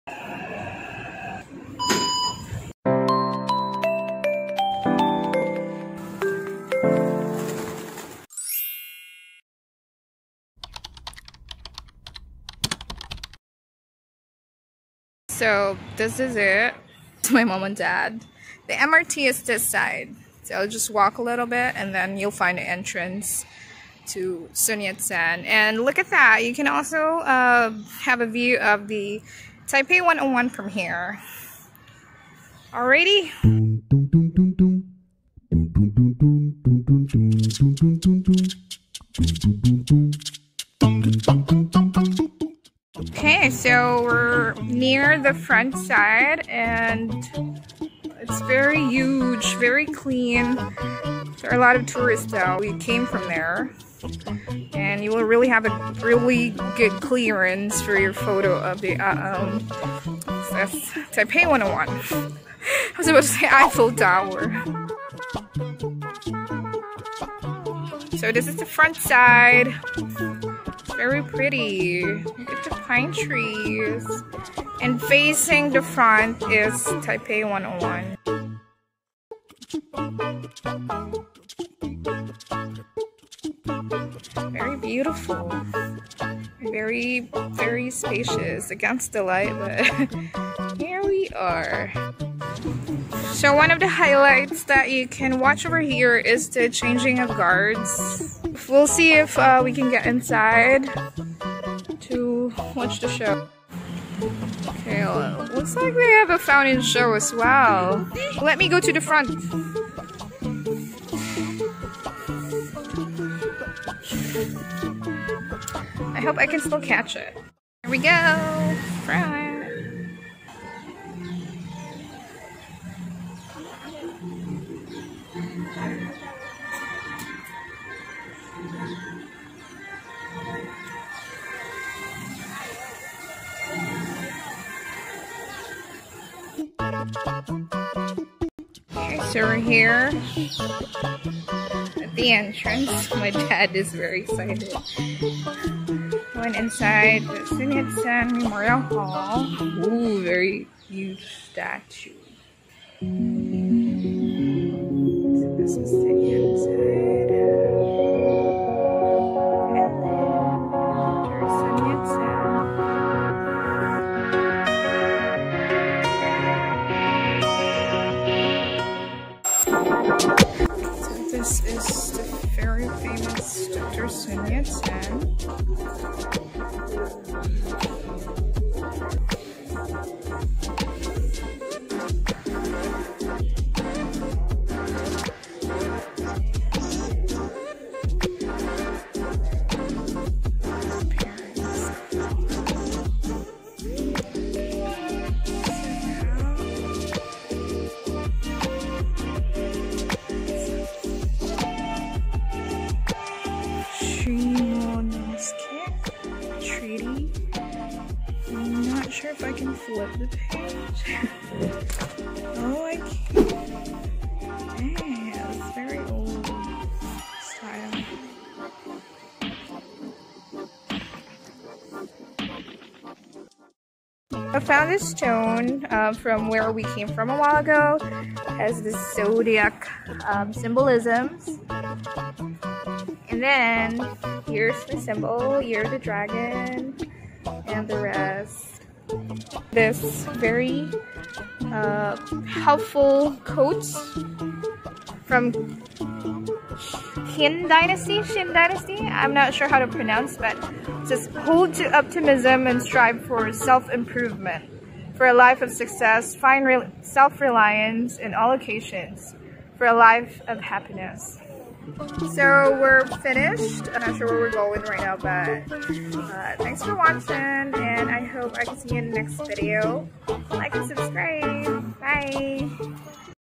How to get to Sun Yat-sen Memorial Hall - Taiwan DIY - Marielle Vlogs
Sharing with you our visit to one of Taipei's historical landmarks. Absolutely no entrance fee.